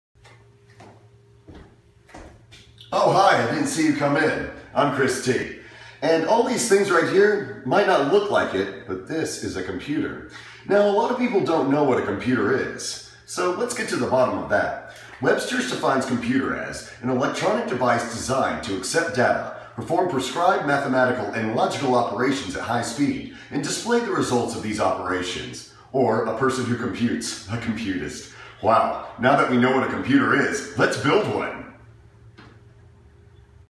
Chris Tergliafera Explains What A Computer Is - And Starts To Build One!
Informational video containing the legal definition of the word "computer"